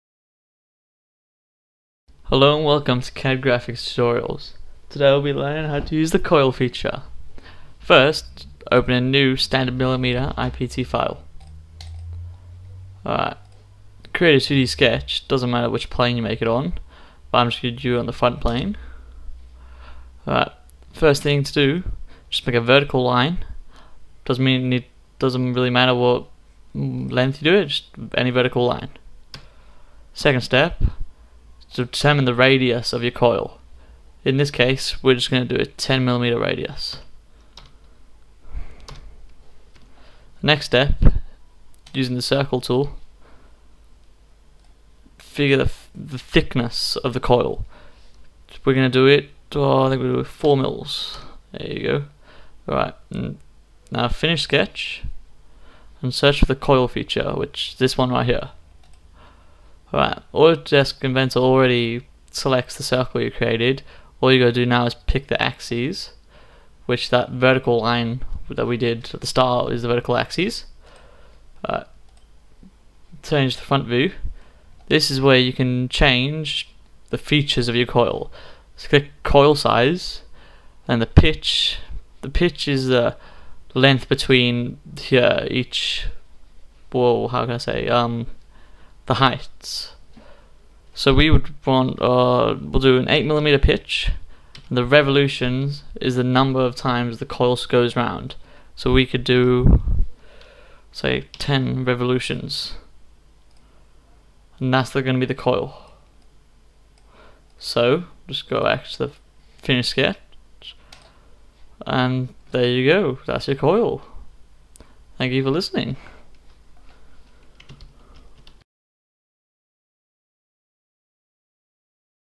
Autodesk Inventor 2014 - Making A Coil / Spring
First of graphics software videos,
How to make a simple coil,
More videos on Autodesk Inventor, Revit and AutoCAD and more Adobe software videos uploading soon
Will have website being published soon